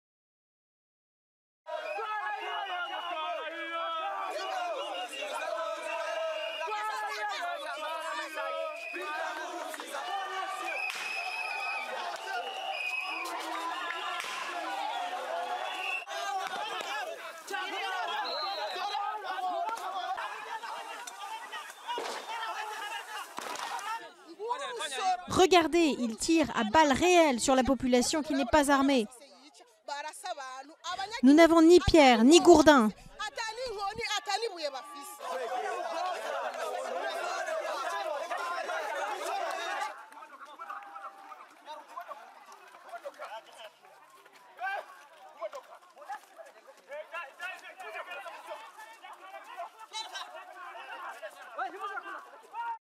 Burundi: un manifestant tué à Bujumbura lors d'affrontements
Un jeune manifestant a été tué mardi 12 mai à Bujumbura où les opposants à un 3e mandat du président burundais Pierre Nkurunziza continuaient de manifester, à la veille d'un Sommet régional dont ils espèrent qu'il convaincra le chef de l'Etat de renoncer à sa candidature. Durée: 00:53